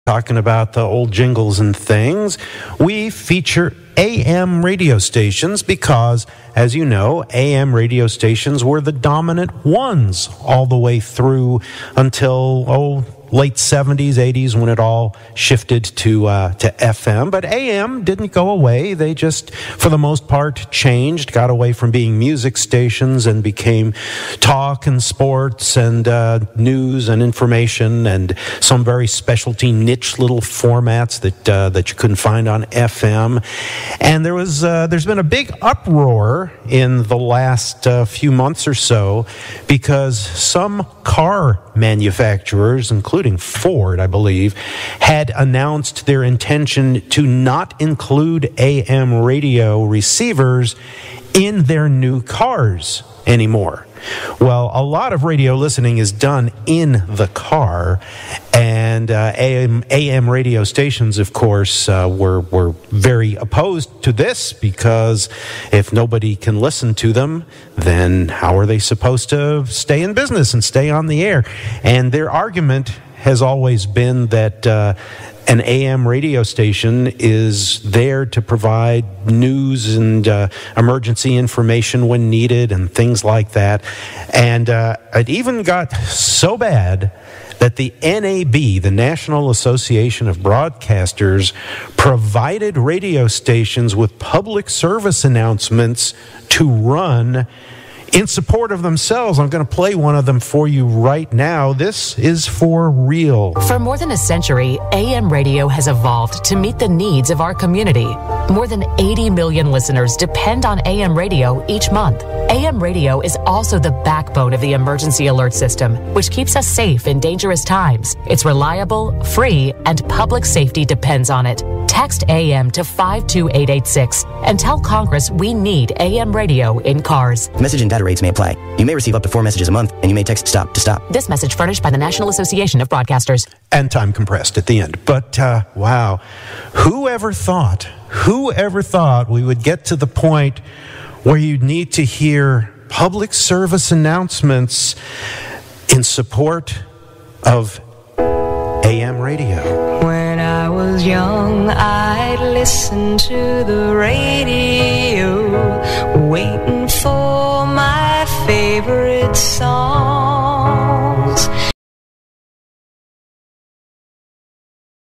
A. M. Radio
This is left over from last weeks, rewound radio Jinko show with John Wolford, here’s more a.m. talk and maybe some more history lessons?